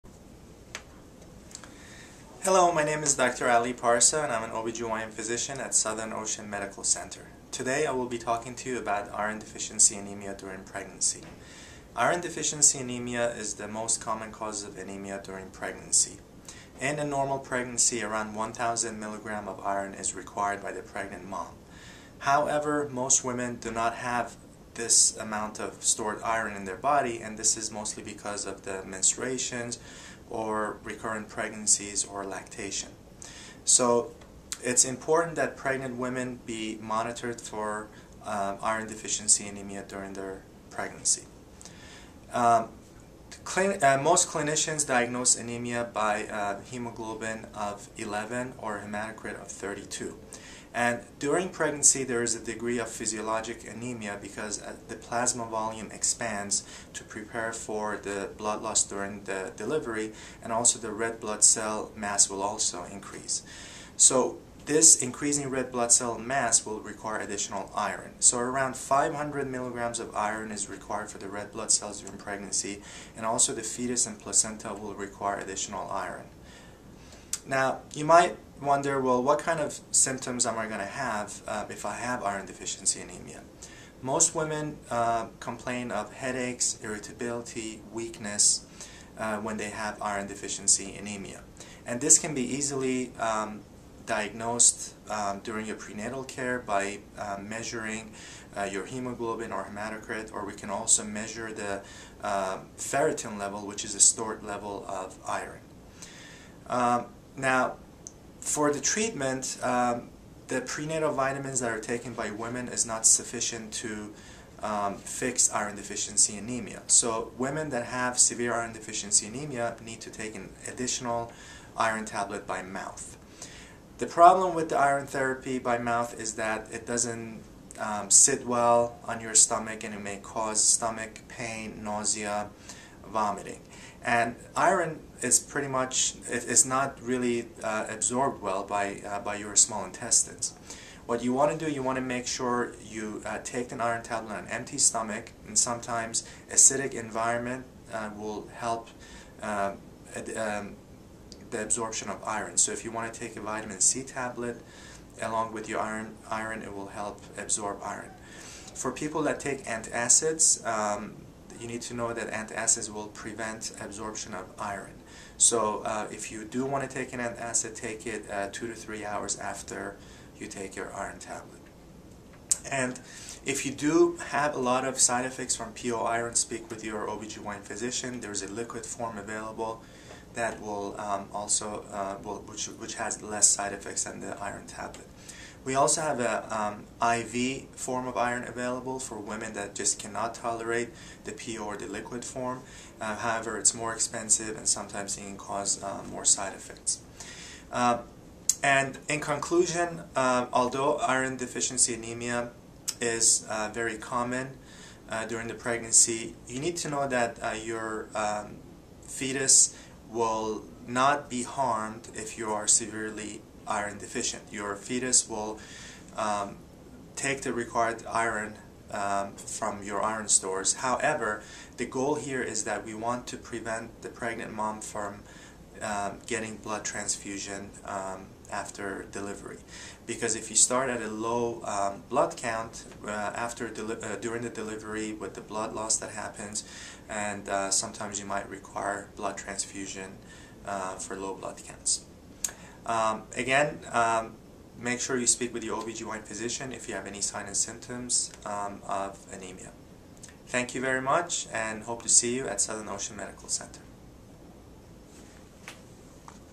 Meridian Momtourage - Iron Deficiency Anemia in Pregnancy -M. Ali Parsa, M.D.
Iron deficiency anemia is the most common cause of anemia in pregnancy. Most women have inadequate iron stores secondary to menstrual blood loss, recent pregnancies and lactation. In a typical singleton gestation, the maternal need for iron averages close to 1000 mg, which exceeds the iron stores of most women.

By M. Ali Parsa, M.D., Southern Ocean Medical Center Obstetrician/Gynecologist